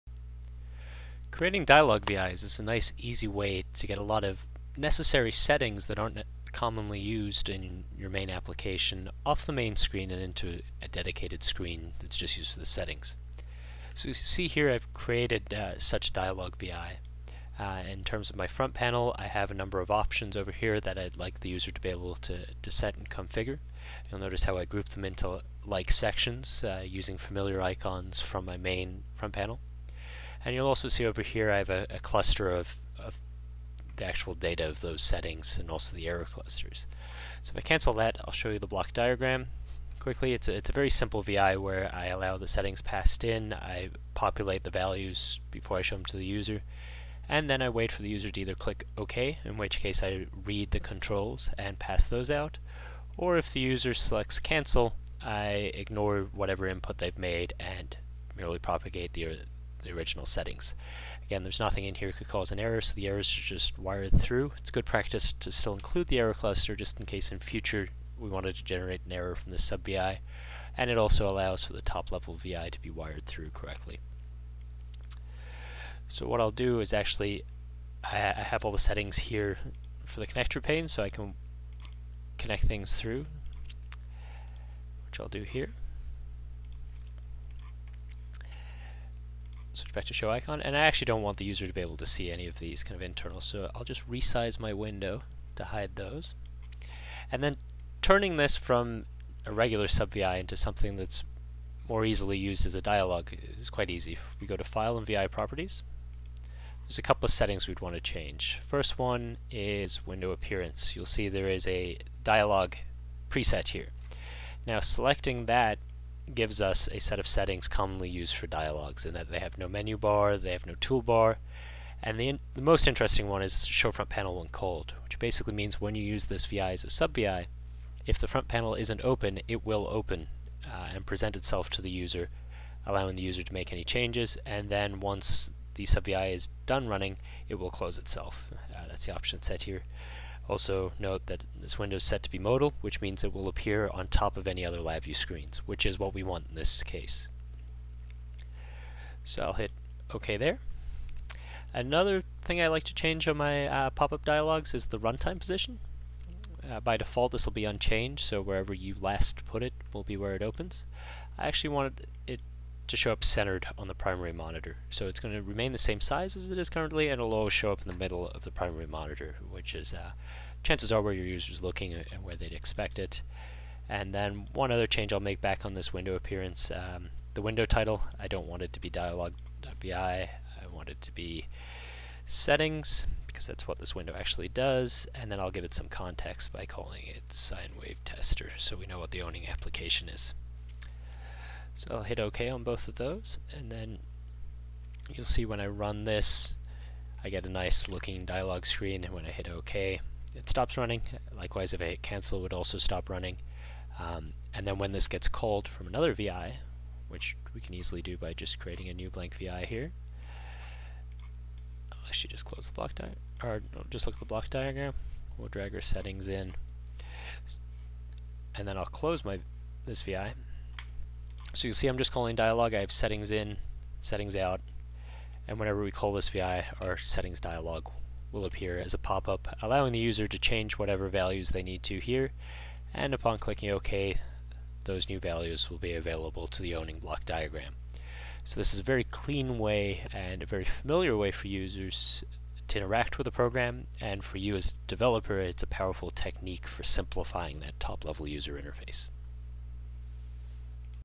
NI LabVIEW UI Tips: Spawn Dialogs
for more UI tips!  Learn how to use dialog Vis and transfer your necessary settings to a separate, easily accessible interface